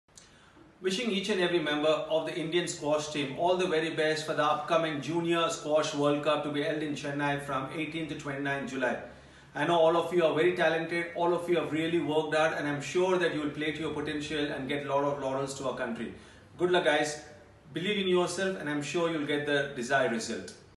VVS Laxman on World Junior Squash Championship 2018
July 18th to 29th, 2018 - Express Avenue is the Official Venue for the upcoming World Junior Squash Championship.